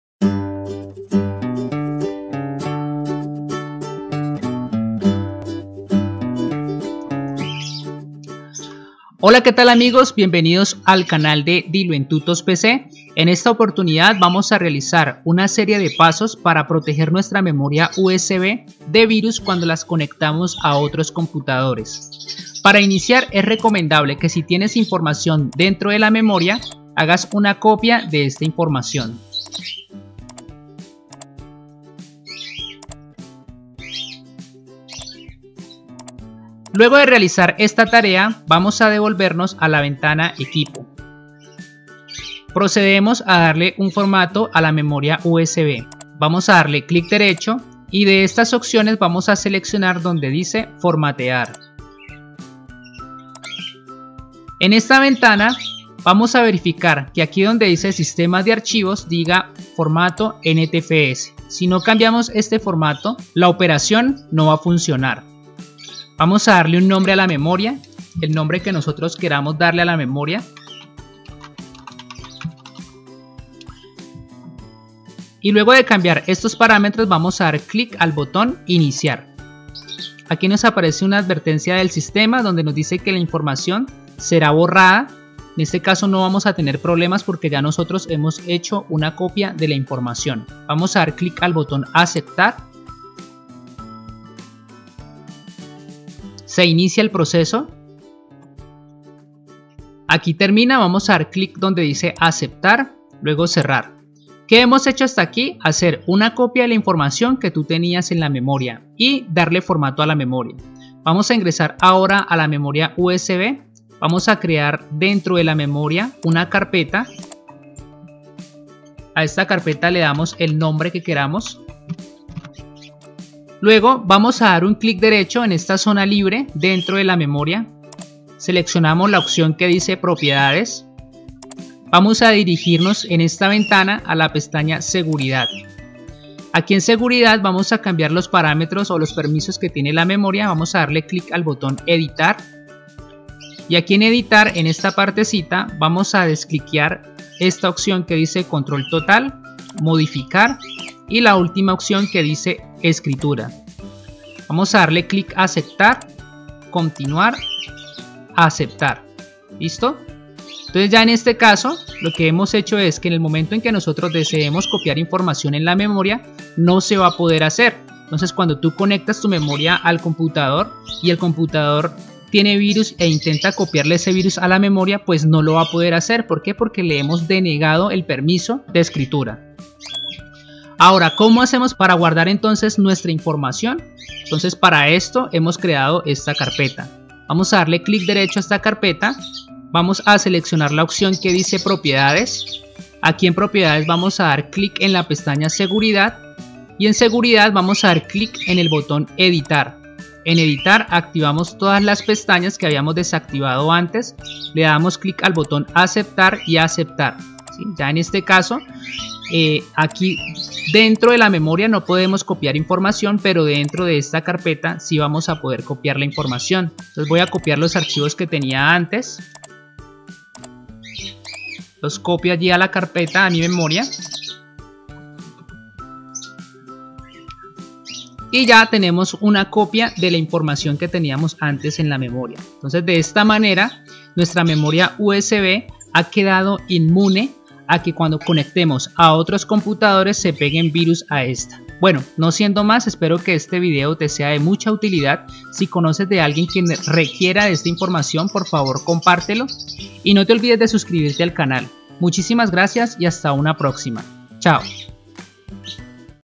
Inmunizar y proteger memorias USB ante cualquier virus 2017 - Sin programas
Hola que tal amigos, en esta oportunidad vamos a realizar una serie de pasos para Inmunizar y proteger memorias  USB ante cualquier virus  2017 - Sin programas

Ya que El 90% de los virus se escribe en la raiz del  USB. Sin embargo  podrìan empezar a hacer los virus de forma que busquen carpetas (quizá ya hay algunos que lo hagan).